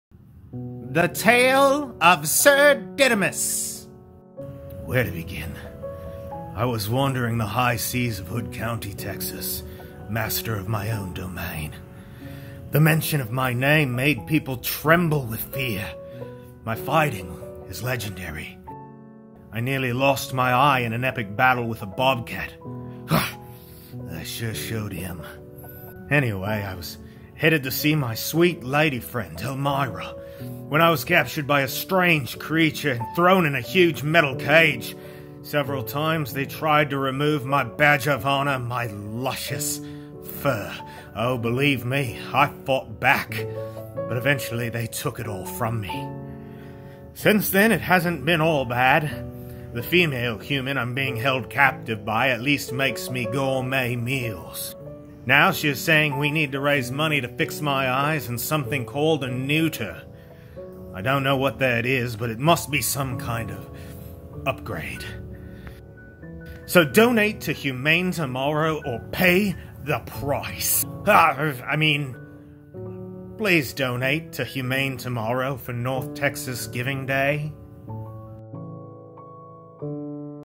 The Tale of Sir Didymus
Action! Adventure! Brunch! Sir Didymus has been through a lot in his short little life. Watch him sip on scotch and tell his tale. to donate toward his medical care.